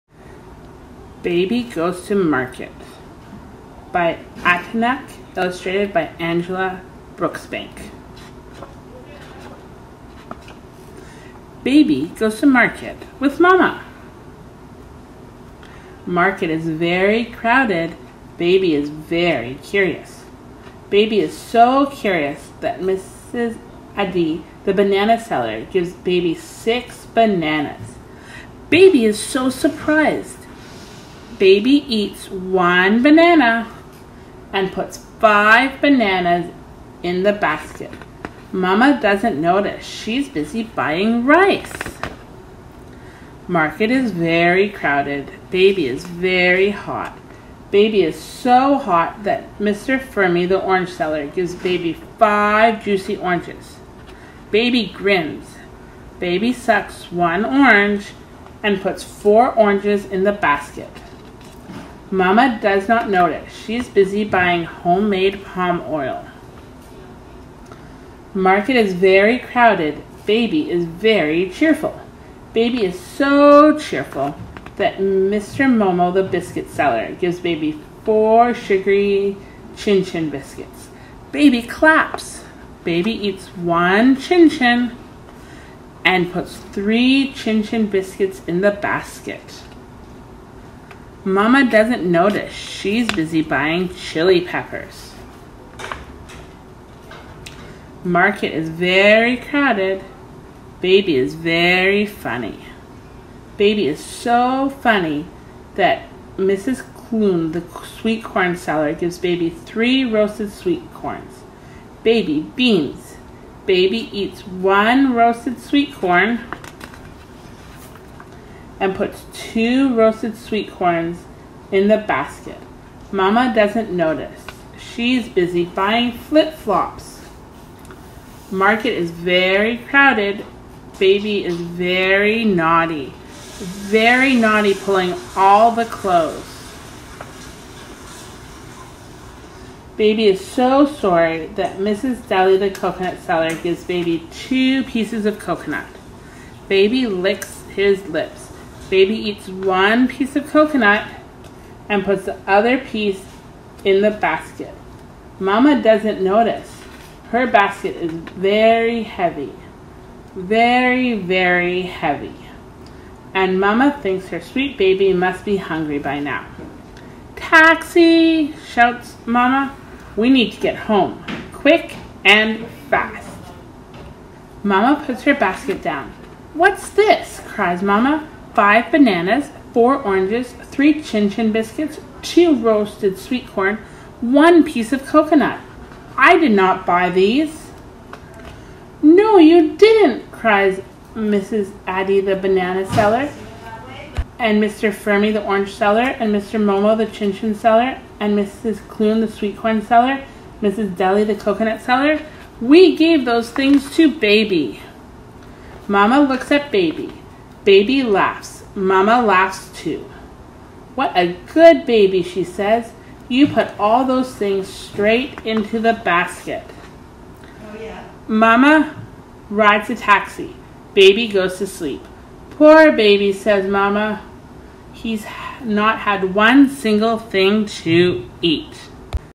Baby Goes to Market
Join Baby and his doting mama at a bustling southwest Nigerian marketplace. What food does baby collect? Find out in “Baby Goes to Market” by Atinuke